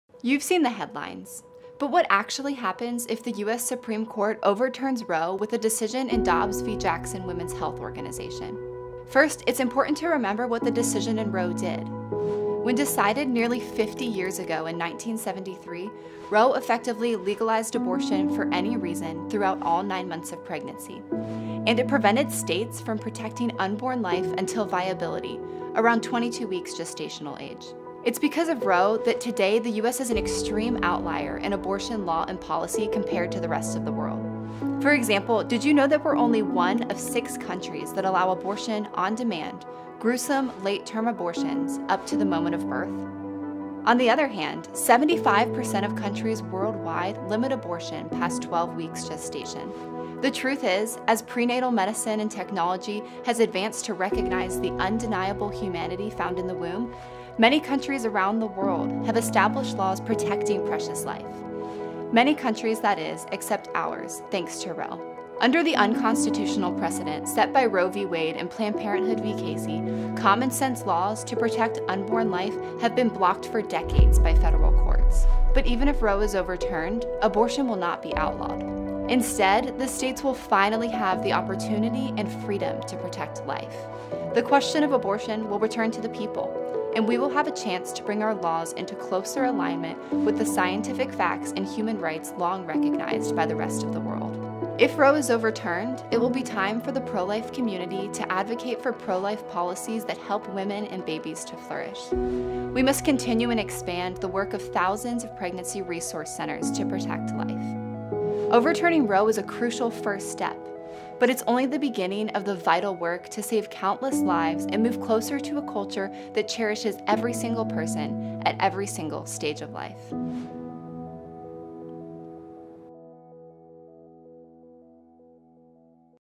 Ce se va întâmpla dacă Curtea Supremă a SUA va schimba Roe v. Wade şi legislația pro-avort?
Ce se întâmplă dacă Curtea Supremă a SUA anulează Roe vs. Wade?

CIF: 33686860